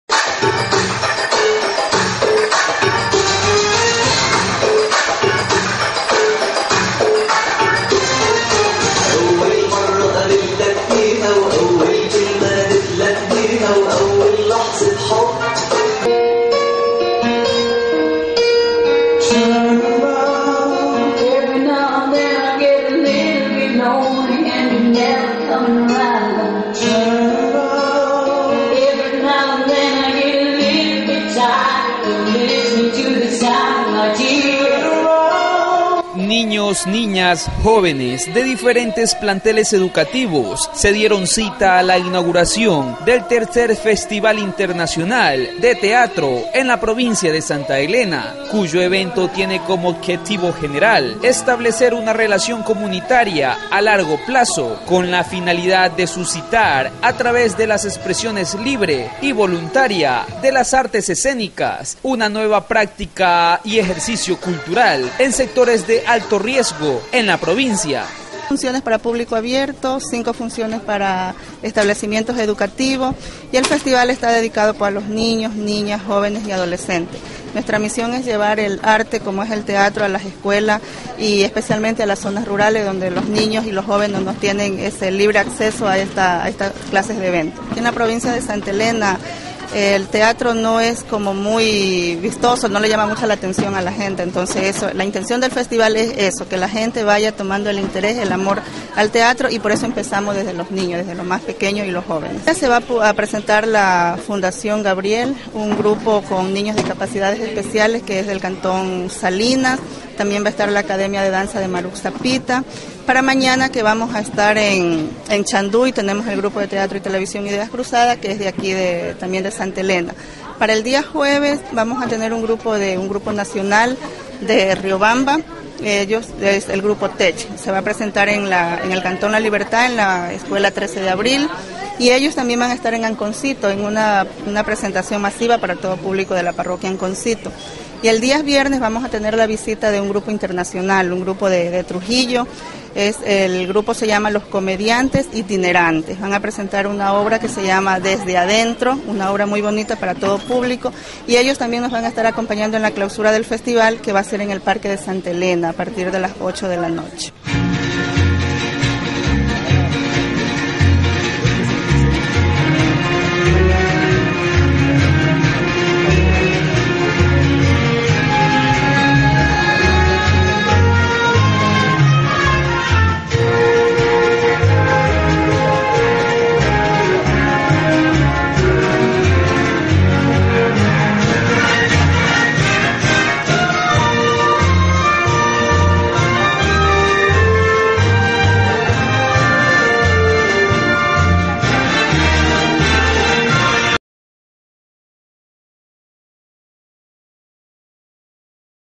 TERCER FESTIVAL INTERNACIONAL DE TEATRO SANTA ELENA 2014
EN EL MUSEO LOS AMANTES DE SUMPA DE LA CAPITAL PROVINCIAL SE EFETUO UNA DE LAS JORNADAS DEL TERCER FESTIVAL INTERNACIONAL DE TEATRO QUE SE DESARROLLA EN NUESTRA PROVINCIA DEL 20 AL 26 DE OCTUBRE DEL PRESENTE AÑO CON EL OBJETIVO DE  ESTABLECER UNA RELACIÓN COMUNITARIA Y ACERCAR AL ARTE A LAS ESCUELA Y COLEGIOS